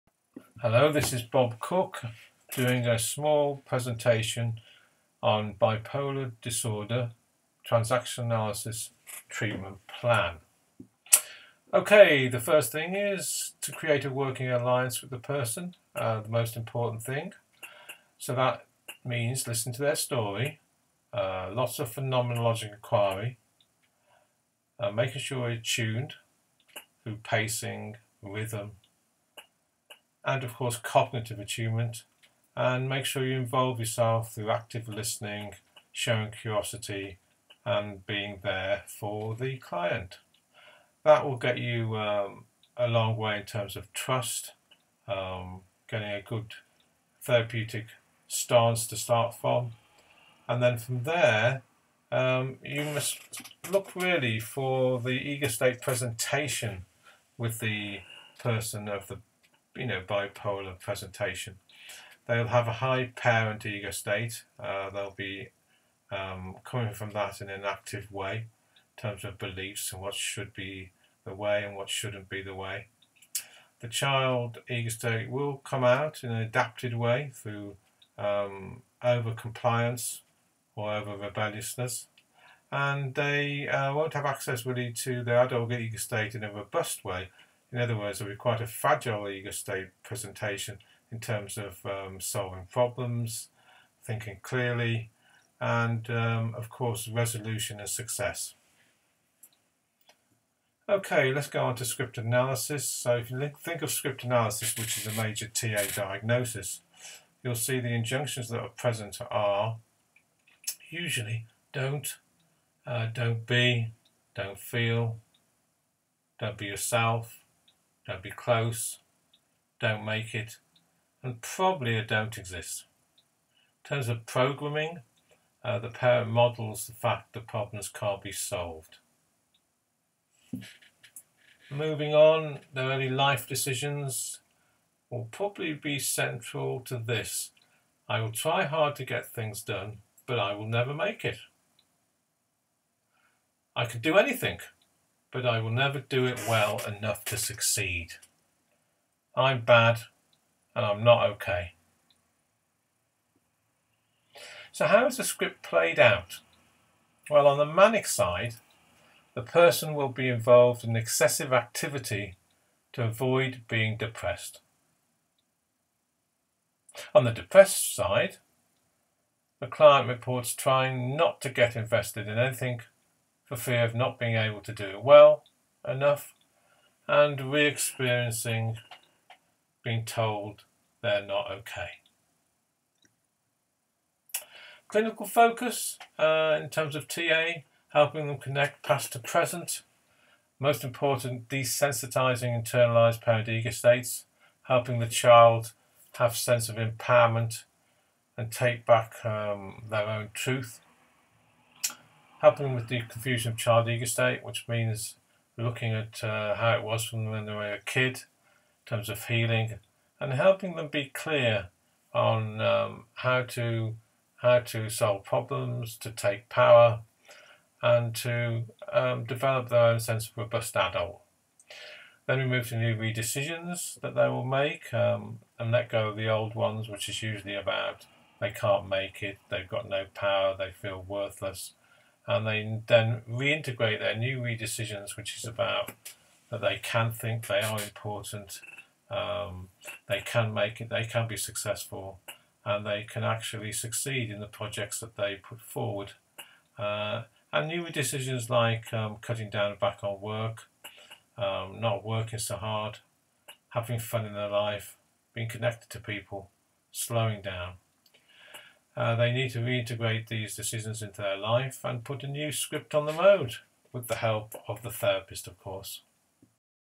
Biploar disorder TA treatment planning
How to work clinically with Bipolar disorder using Transactional Analysis